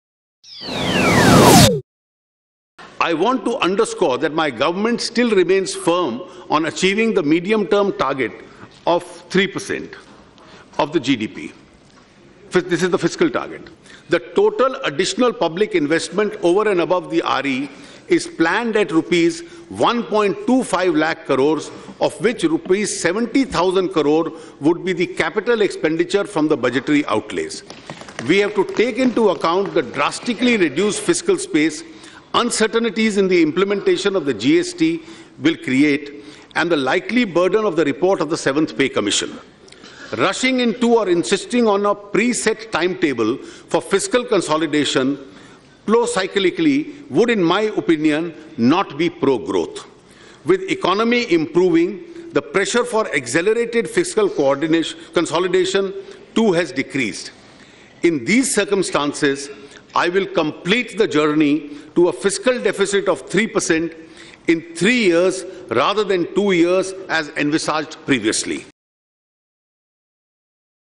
‘Medium Term Fiscal Target of 3% of GDP’
Economic growth this year at 11.5 percent is lower in nominal terms but will meet the challenging fiscal target of 4.1 percent of GDP. Jaitley further said that fiscal deficit target of 3 percent will be achieved in three years rather than the two years announced earlier.